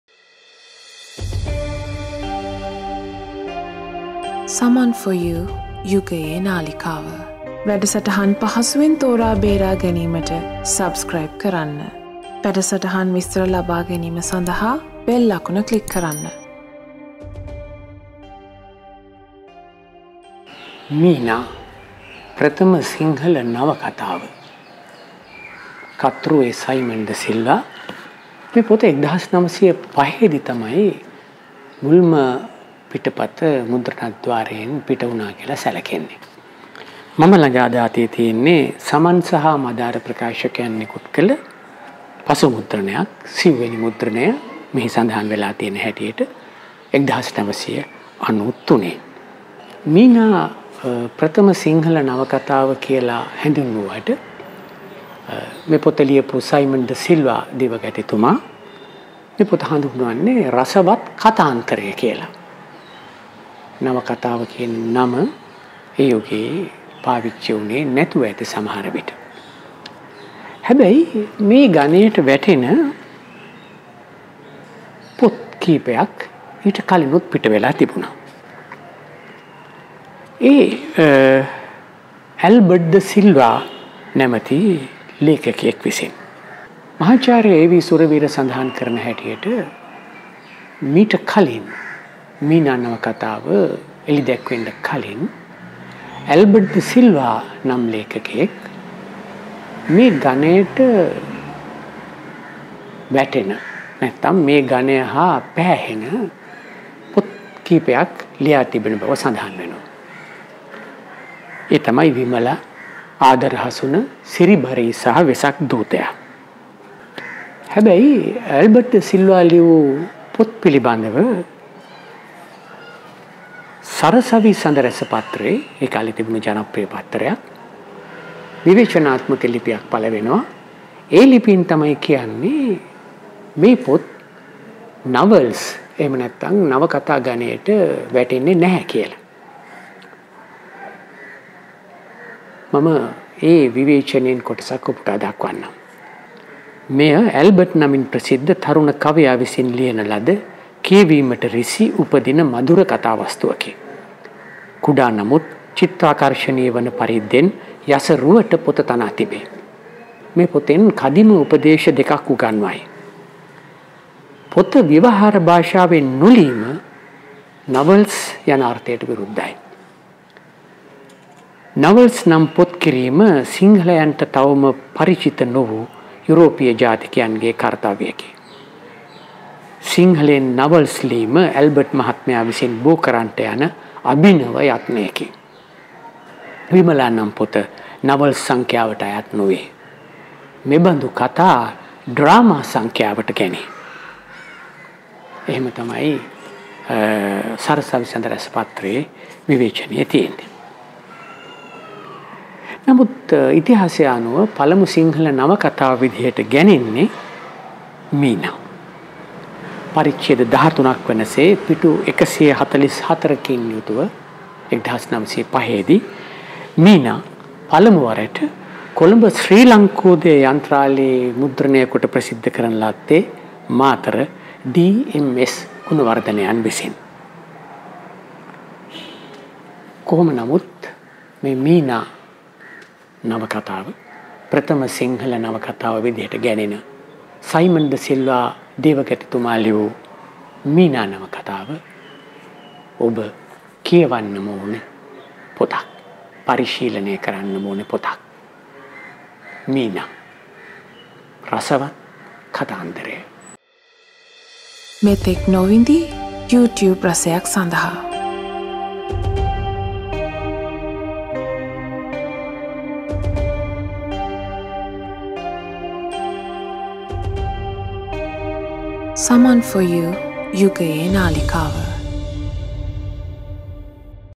Saman4You - Prog 127 - මුල්ම සිංහල නවකතාව මීනා | Meena | First Sinhala Novel
ඔබ මීනා කියවලා තියෙනවා ද? මීනා ගැන දන්නවා ද? සිංහල පතපොත ගැන උනන්දුවක් දක්වන අය අනිවාර්යයෙන් ම මීනා ගැන දැනගත යුතු යි. මීනා කියවිය යුතු යි. මුල් ම සිංහල නවකතාව සේ සැලකෙන්නේ මීනා කෘතිය. අකුරක රස වැඩසටහන් මාලාව මගින් මීනා ගැන දැනගන්න. මීනා කෘතියට සිංහල සාහිත්‍ය වංශ කතාවේ හිමිව ඇති ස්ථානය ගැන අවබෝධ කරගන්න. Books 4 You